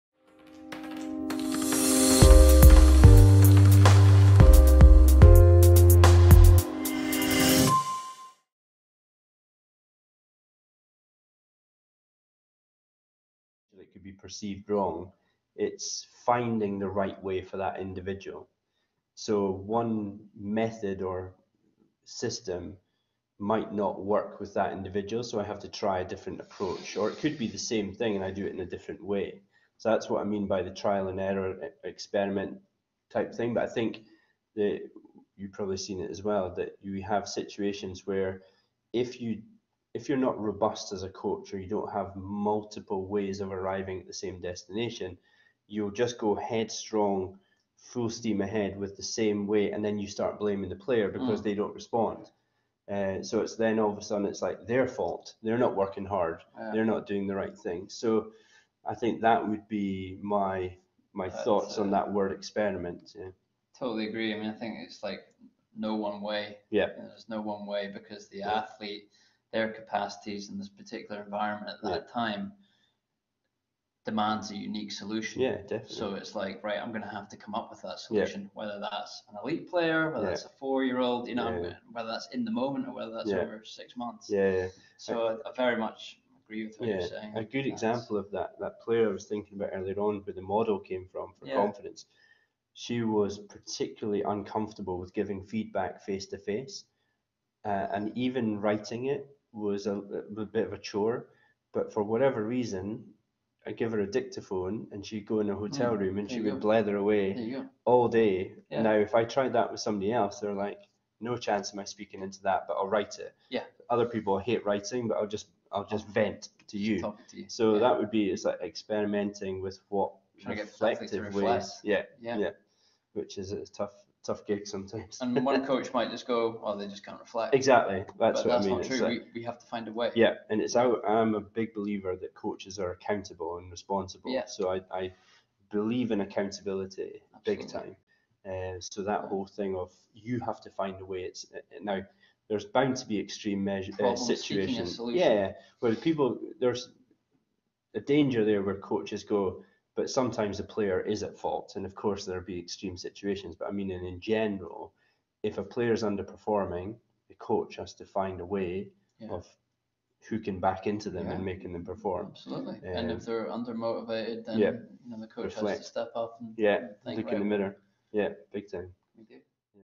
FIND A WAY FOR YOUR PLAYERS TO GET BETTER
In June 2019, I was asked to be a guest on a webinar at Robert Gordons University in Aberdeen. I was joined by Neil Buchanan, who heads up the Applied Sport and Exercise Science Degree at the university.

I have chopped the footage up into 10 clips and in this clip you will hear why I believe it is up to us coaches to find ways to help our players progress